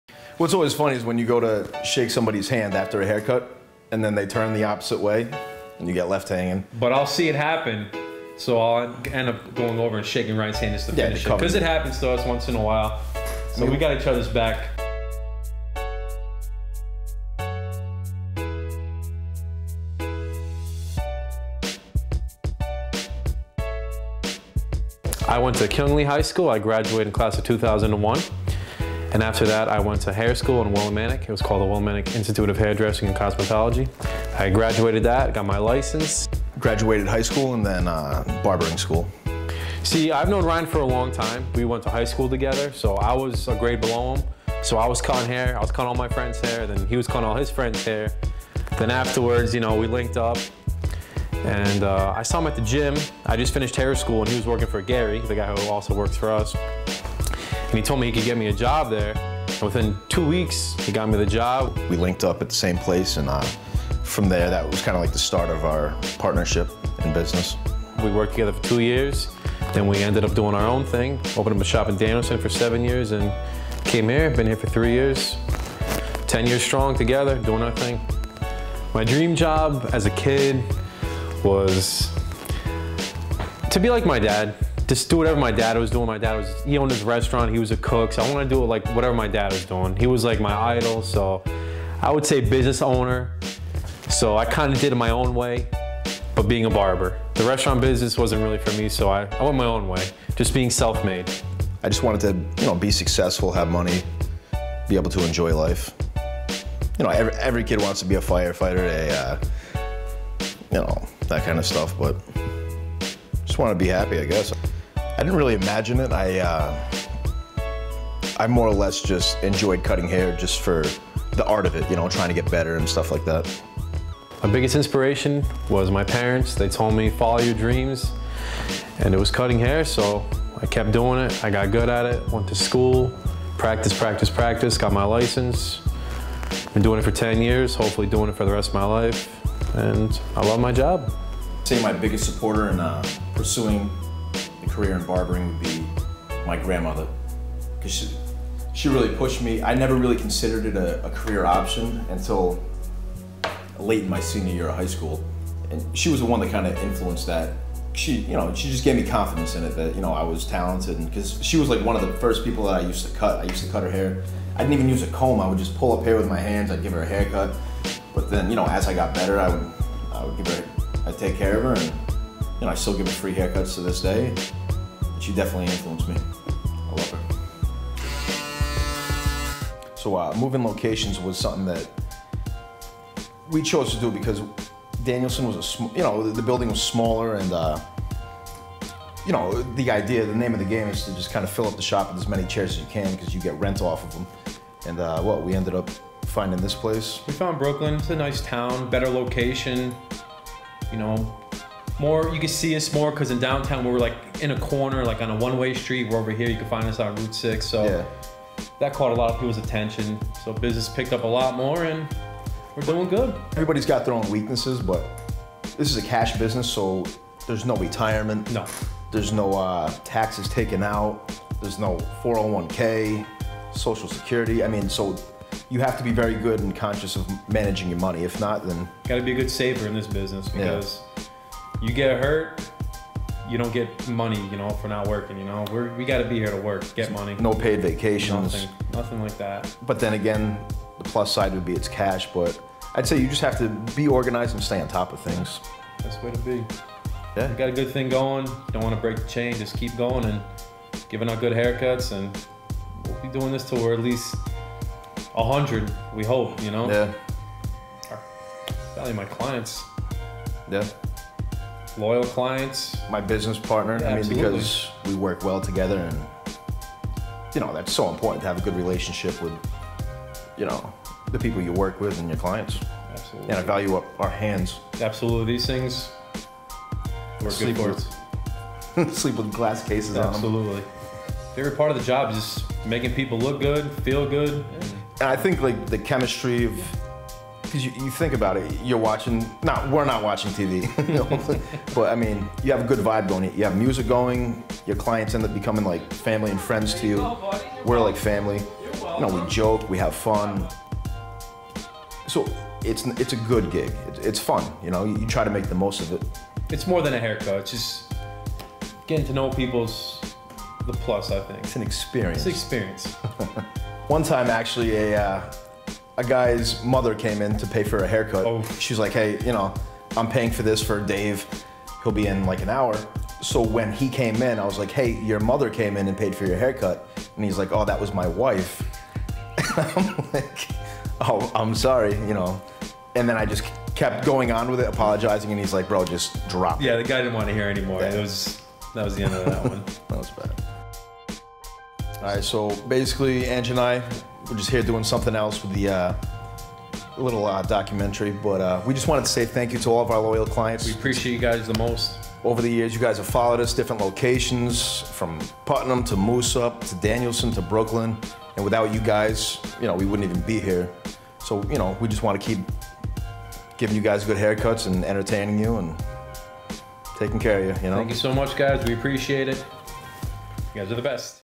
The Barber Shop (Documentary)
A short Documentary highlighting the success of Angelo and Ryan. Directed by Senad Otajagic. All music owned by DJ Burnz and Lesson